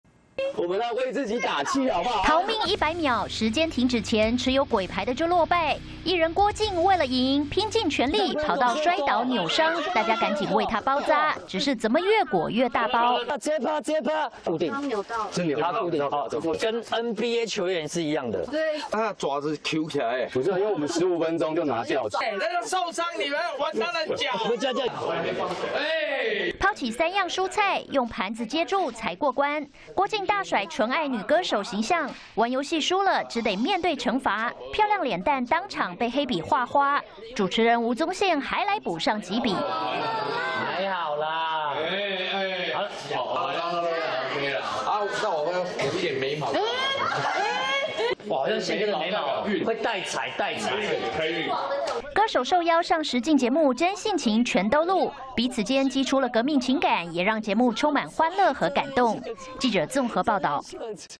"玩很大"輸到底 郭靜慘被畫成大花臉│中視新聞 20220430
「綜藝玩很大」全新一季戰況進入白熱化，藝人郭靜為了贏，跑到整個人摔倒扭傷，玩輸遊戲，還被畫成大花臉。